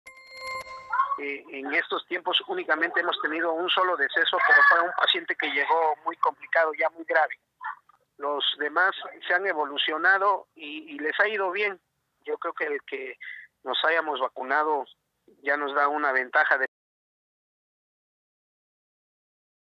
BITE 02 HOSPITAL JUCHITAN COVID
Señaló que en estos últimos días solo se ha tenido un paciente fallecido a causa de esta enfermedad, ya que la gran mayoría logra superar la crisis de la enfermedad, y además indicó que las vacunas han ayudado mucho a la recuperación efectiva de las personas contagiadas.

Este hospital actualmente cuenta con 4 camas para adultos y una cama pediátrica, que se destinan exclusivamente para pacientes positivos de Covid-19, que requieren de atención y cuidados especiales.